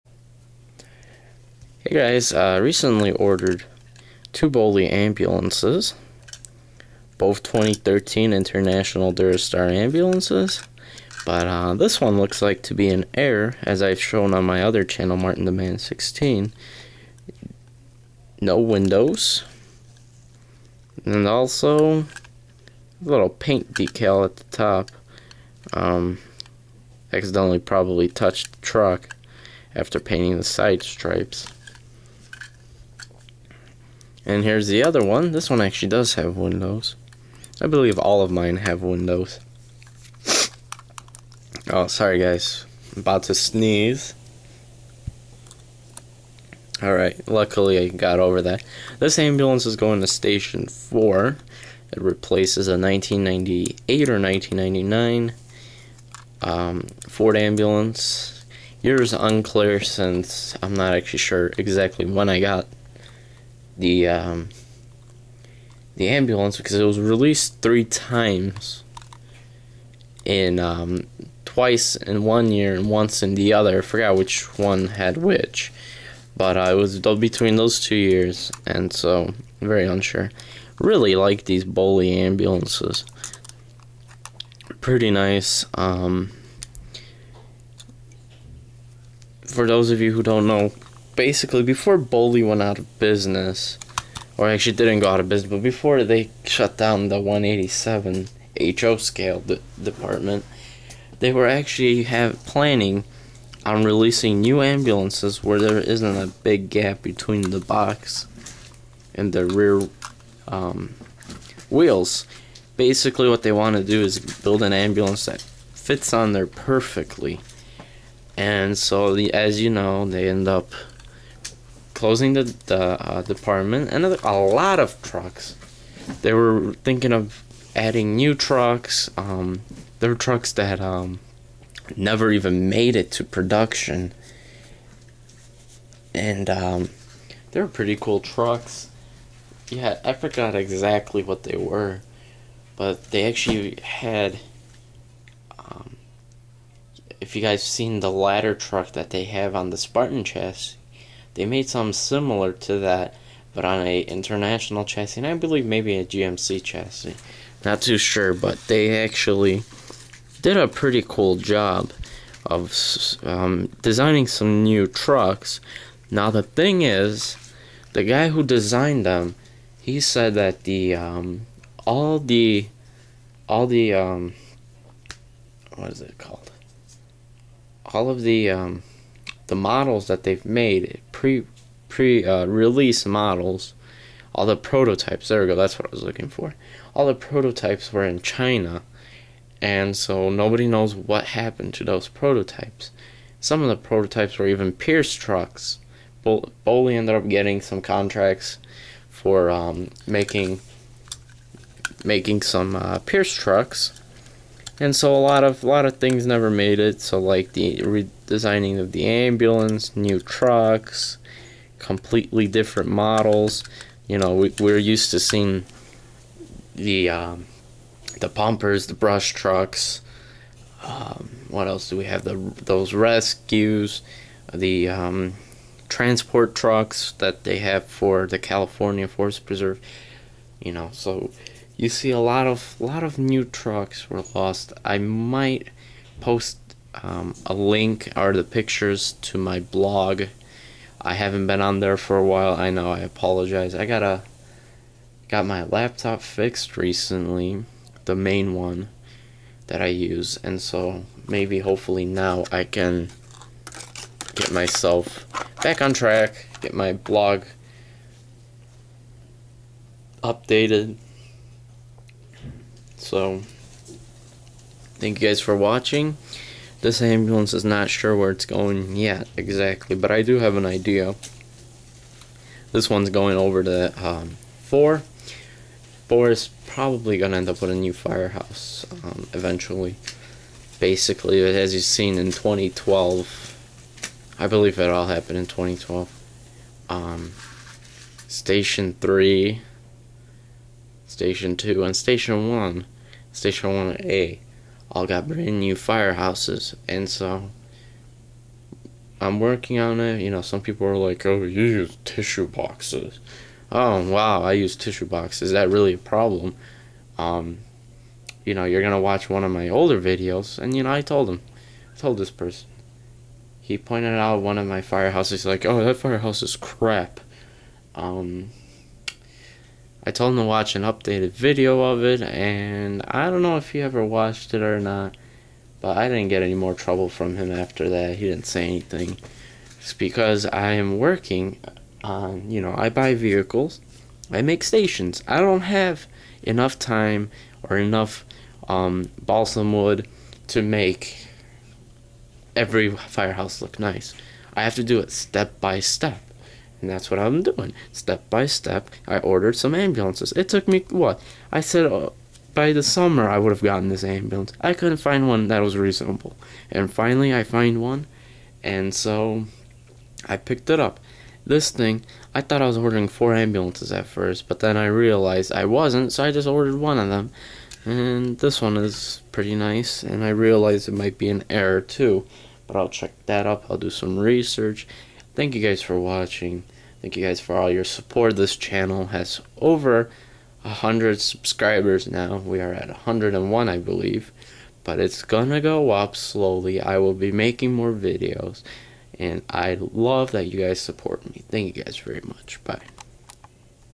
Boley International Ambulances for MBFD
Matchbox Fire Department has taken delivery of two brand new 2013 International Durastar Ambulances